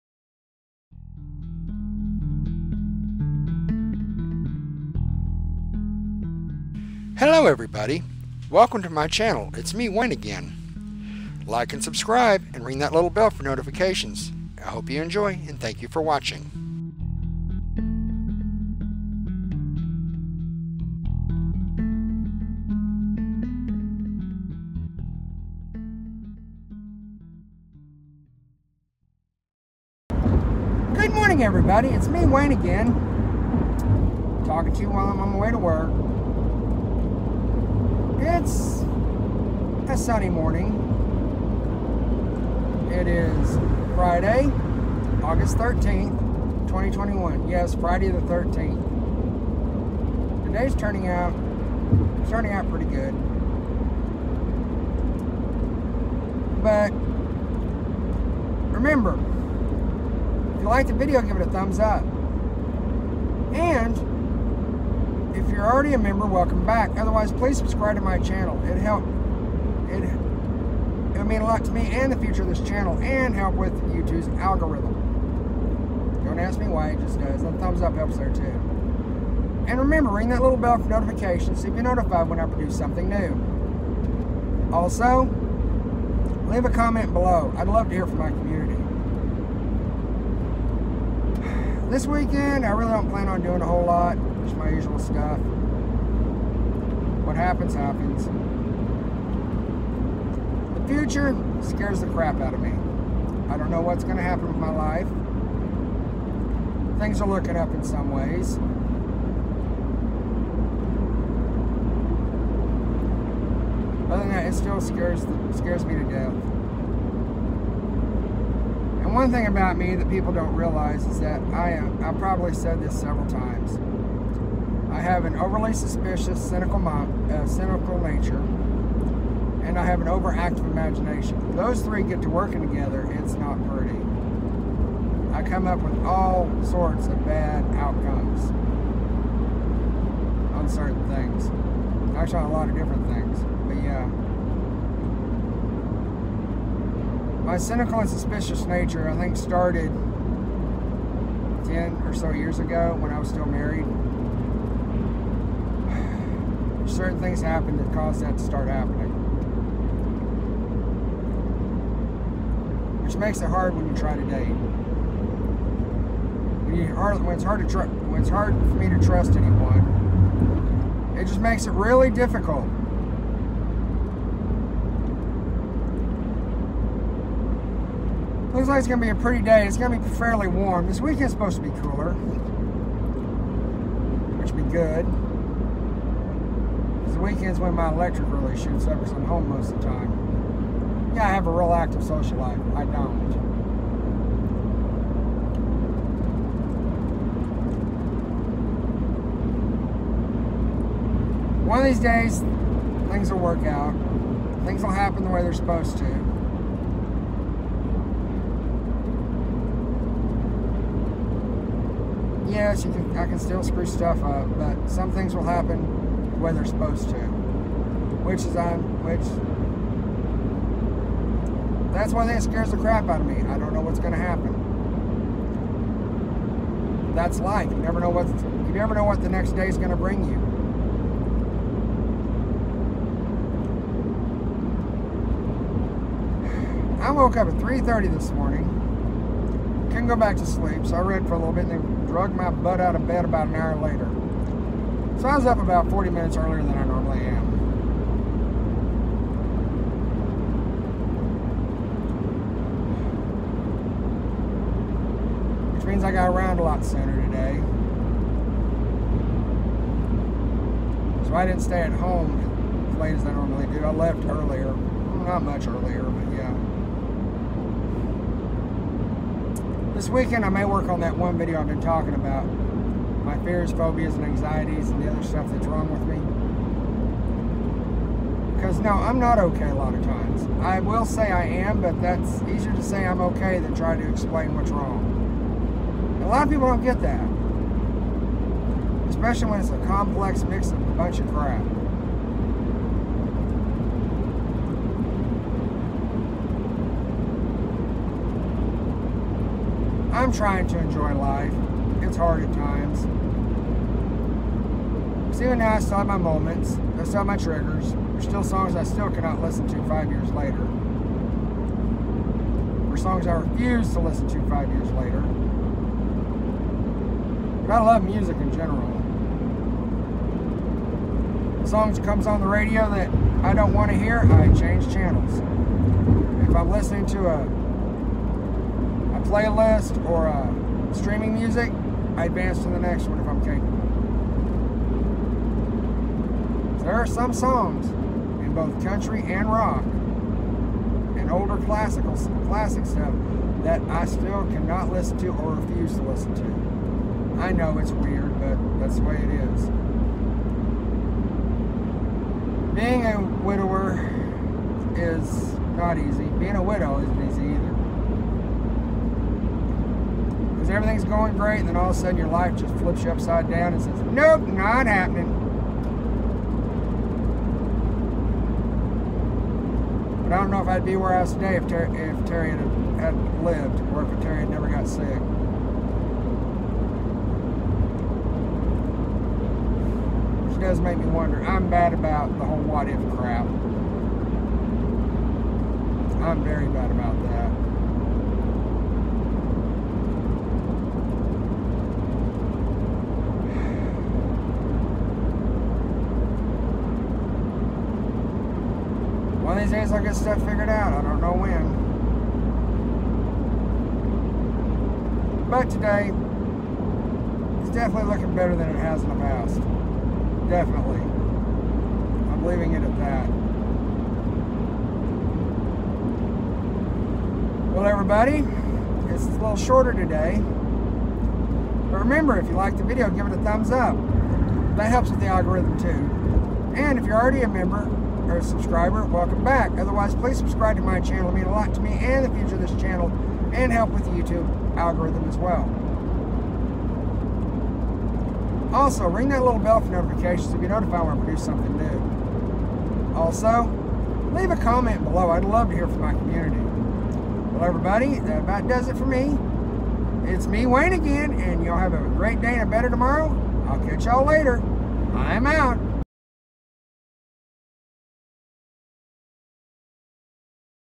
It's me Wayne, Again! A widower's journey part 22.
This is just my normal end of the week update.  Nothing out of the ordinary this weekend other than I plan on working on the video I have been talking about doing.

this has some of my random ramblings.

Somethings in my life are looking up but we shall see,